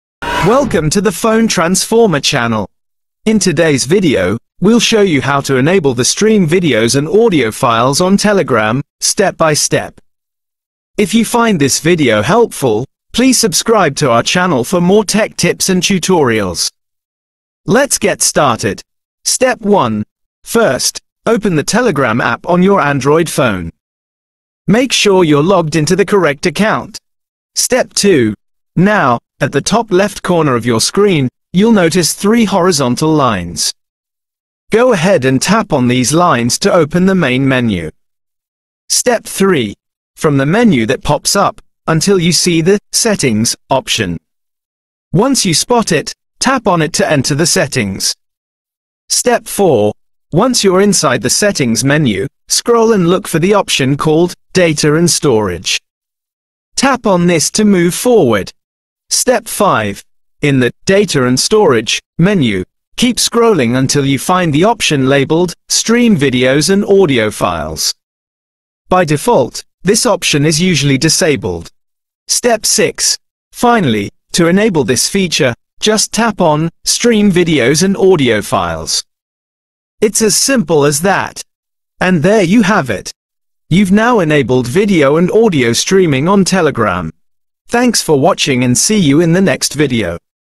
How to Enable the Stream Videos and Audio Files on Telegram
Streaming videos and audio files on Telegram allows you to enjoy your media without needing to download them first, This feature makes it easy to share and view content directly in your chats, providing a smoother experience for users who want instant access to media files, Whether you're sharing a video with friends or listening to music, enabling this feature enhances your overall Telegram experience, In this video, you'll find a step-by-step solution on how to enable the stream videos and audio files on Telegram, making media sharing effortless

- Stream media on Telegram
- Enable audio streaming Telegram
- Watch videos directly on Telegram
- Telegram media settings
- Share audio files on Telegram